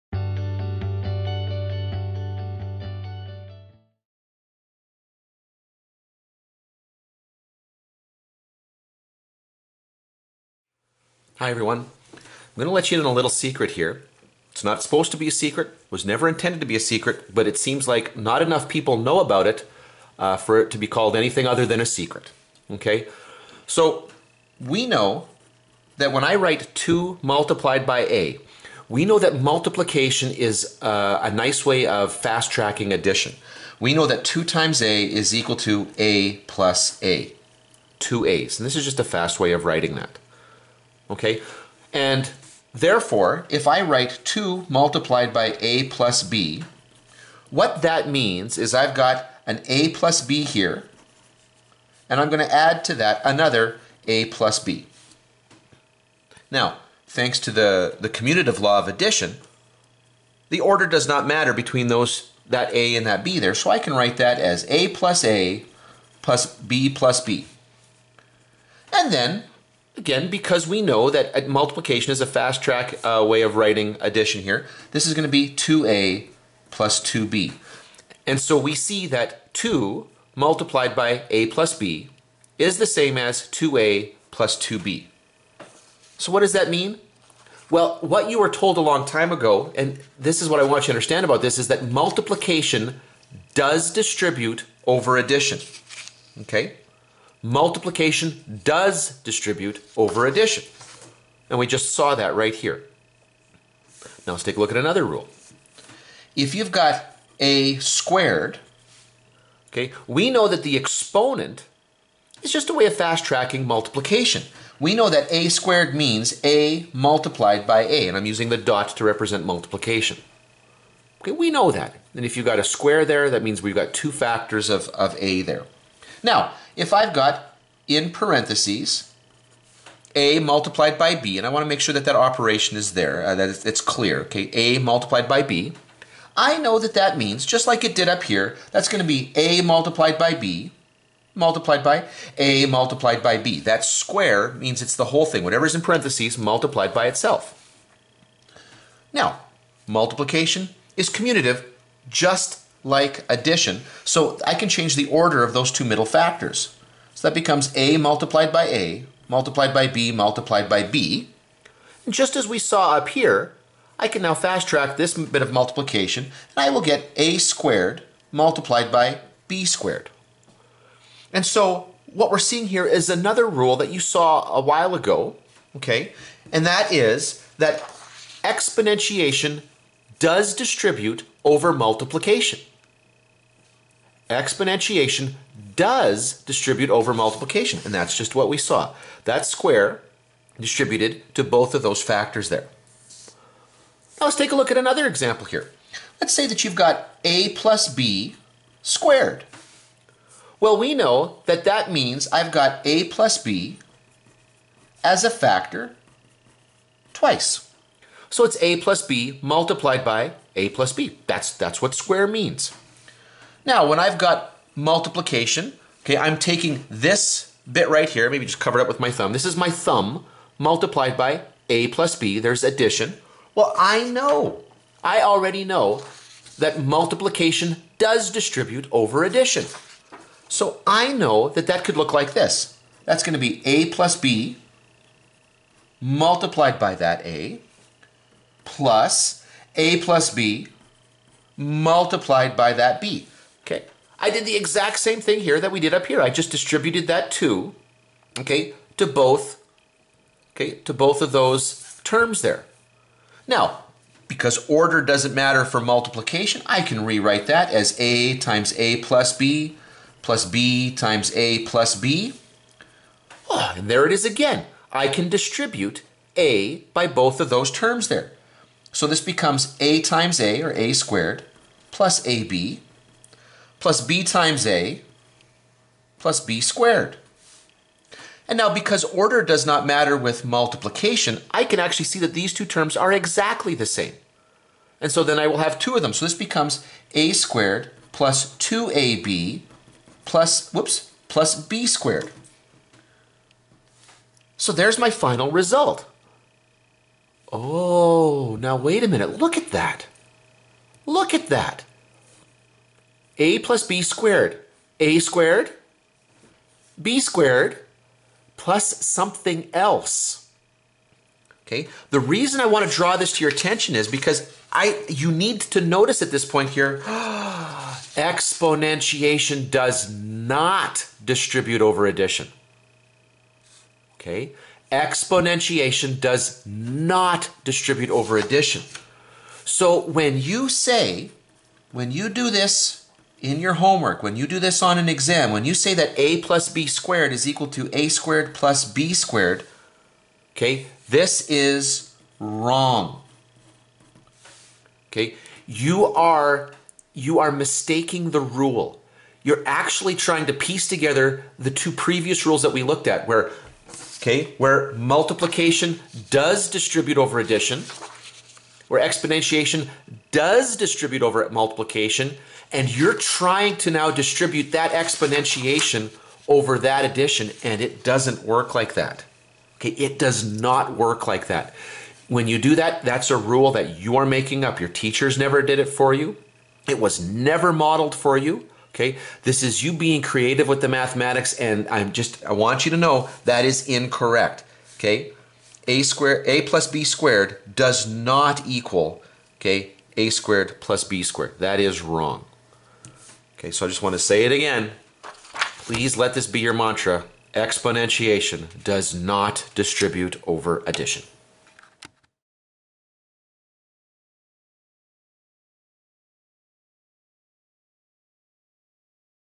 Exponentiation does NOT Distribute over Addition
This video is intended to help people understand the distributive property.  The specific issue it is intended to address is the common mistake of distributing an exponent across the terms of a polynomial.